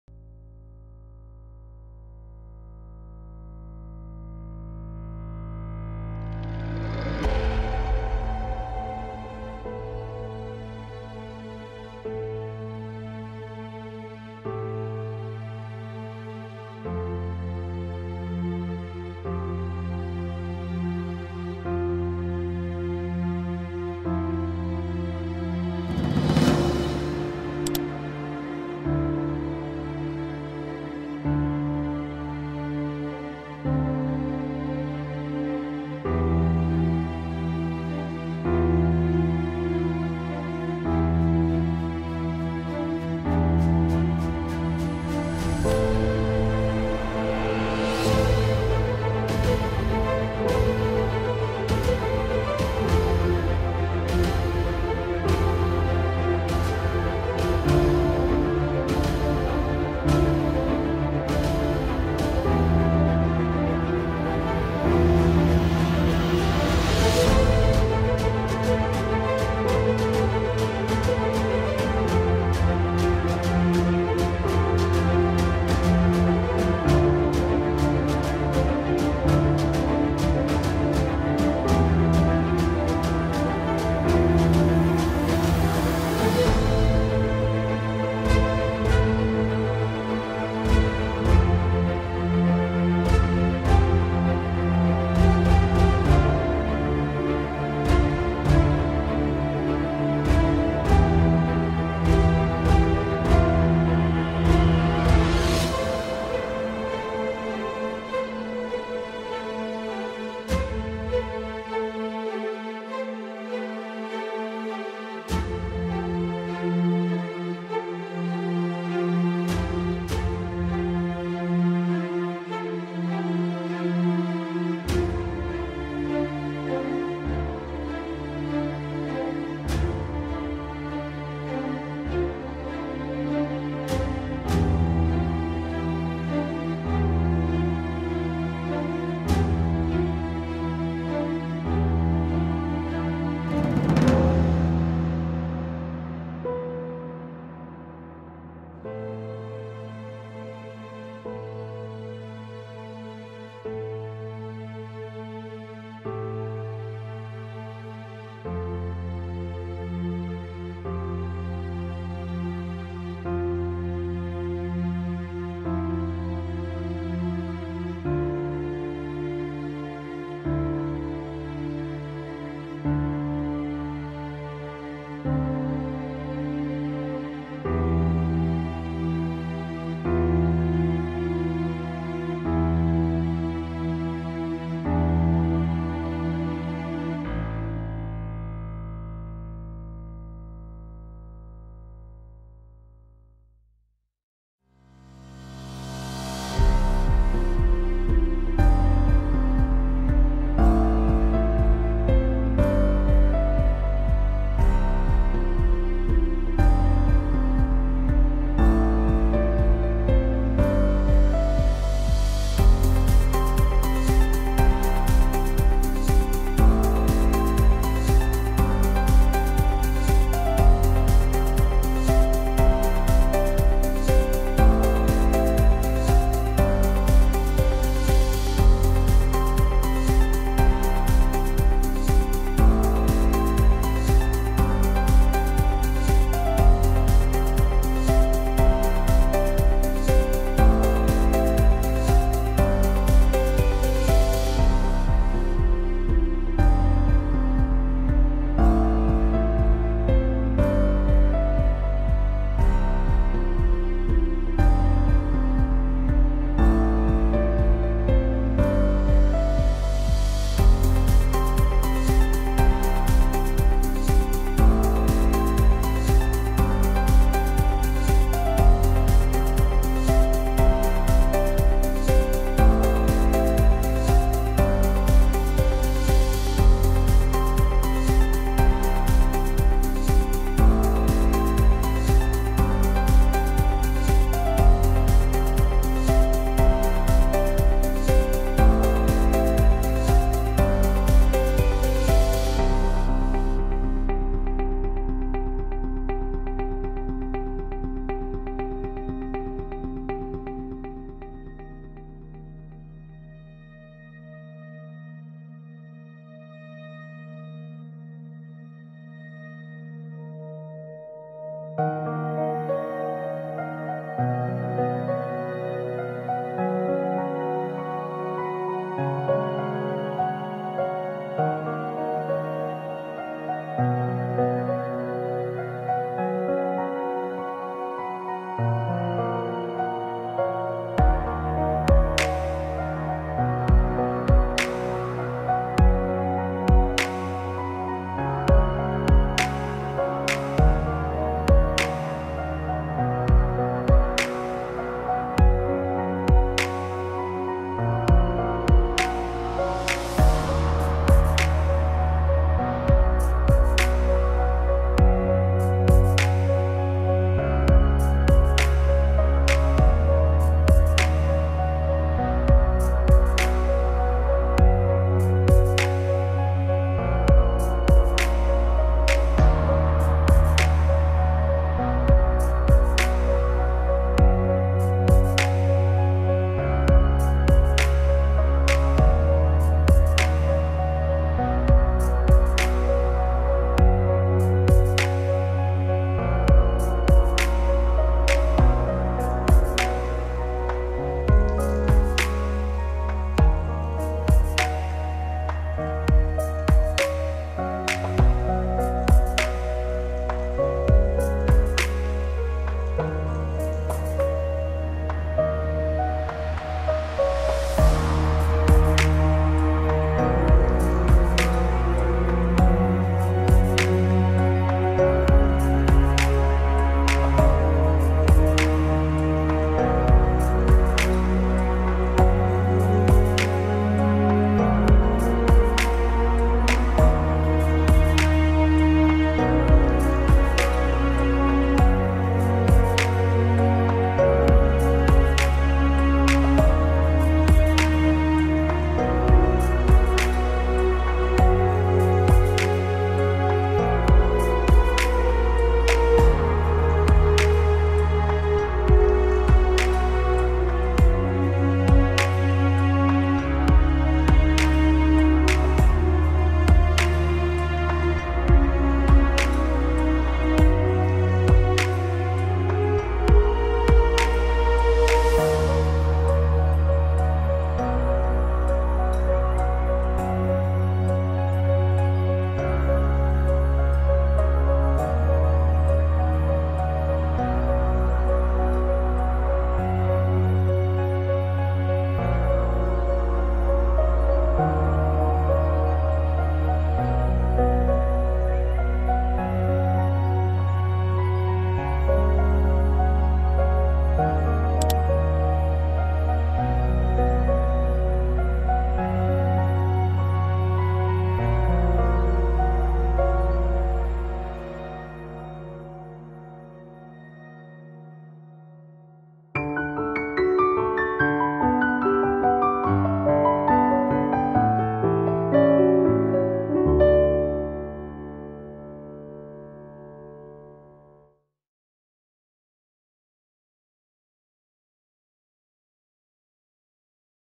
「アフターエフェクト」チュートリアル64 3D フォトスライドショーの作り方 | 速度調整 イージーイーズイン・アウト
チュートリアル動画

4K スライドショーの作り方 56    効果  | CC Particle World・Transition・被写界深度